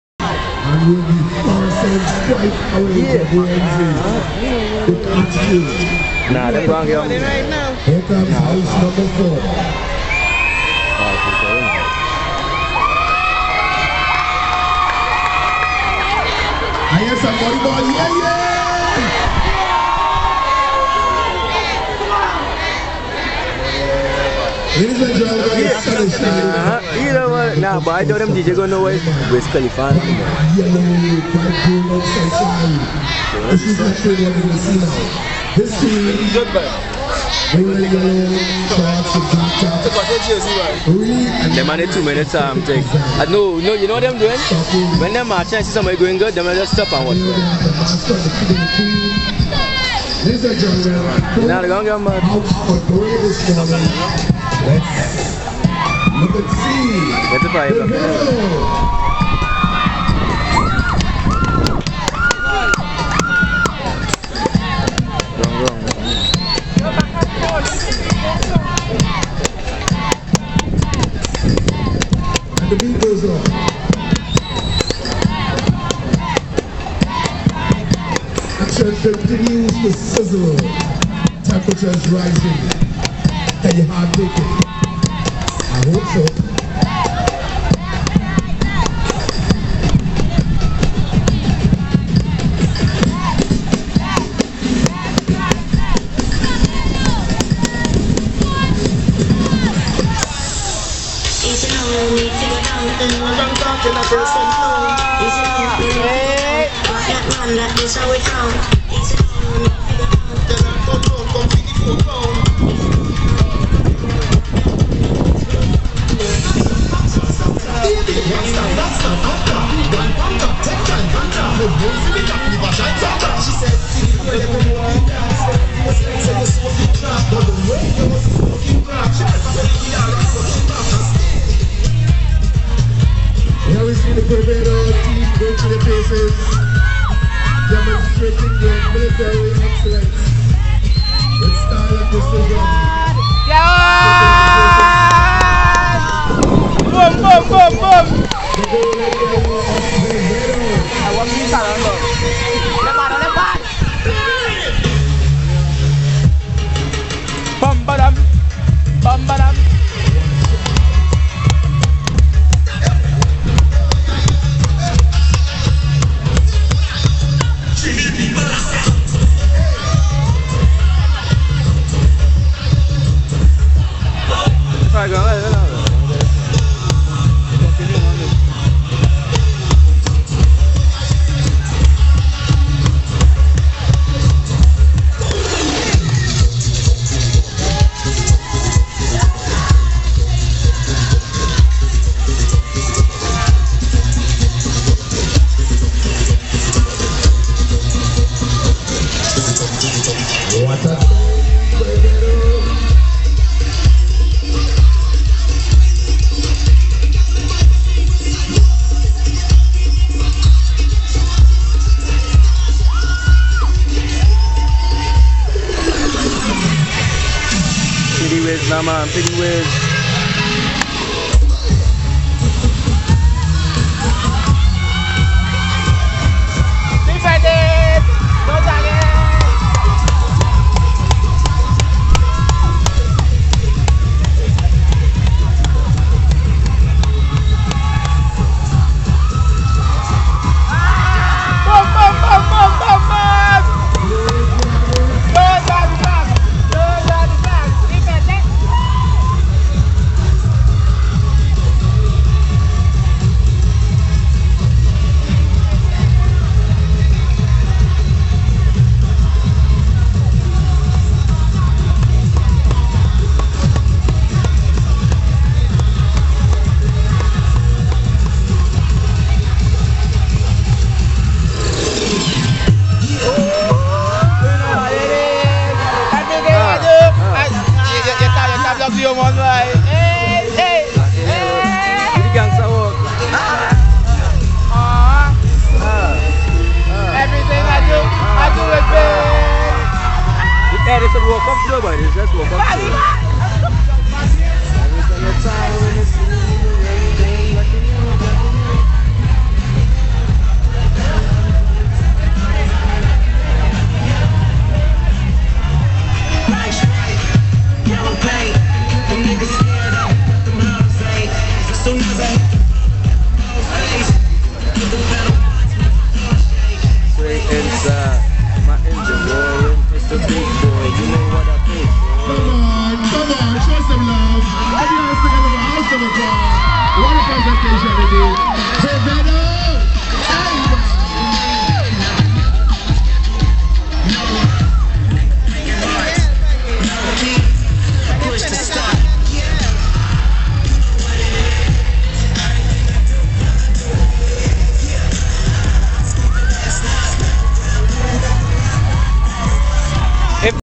Sepos Marching. Quevedo *3rd Place*
Quevedo marching in their black and yellow colours